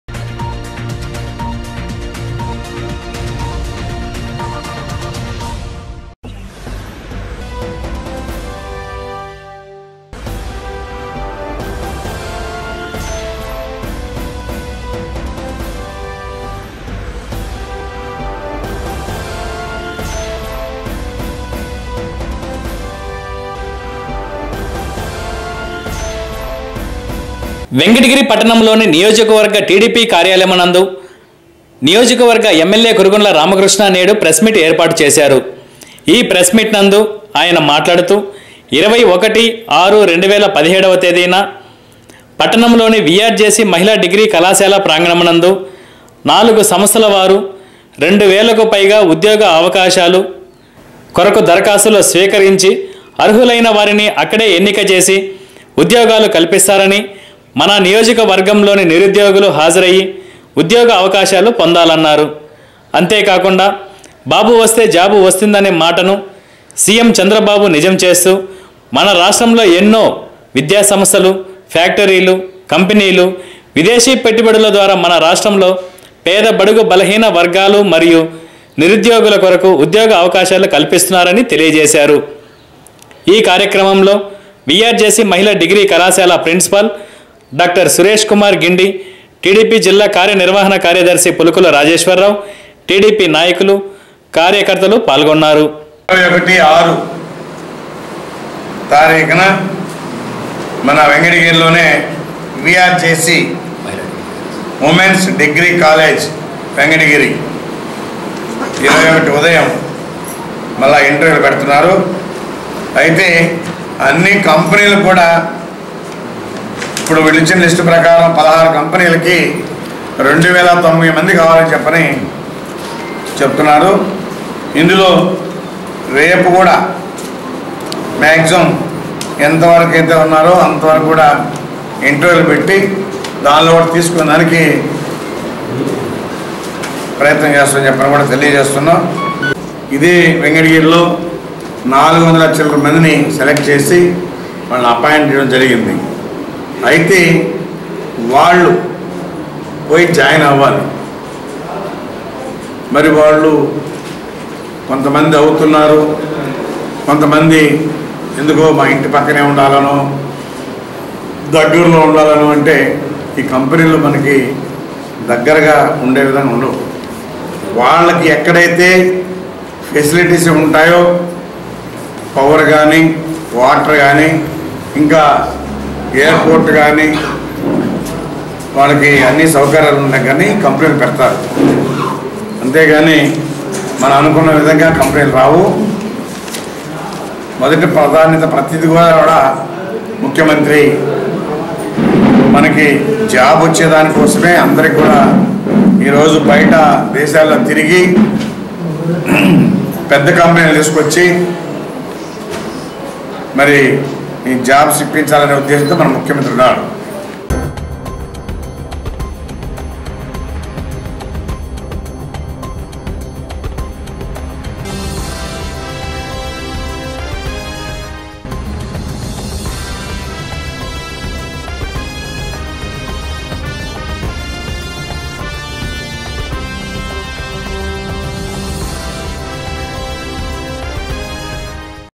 జేపిన్యూస్ వెంకటగిరి
వెంకటగిరి యం.ఎల్.ఎ అధ్వర్యంలో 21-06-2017 జాబ్ మేళ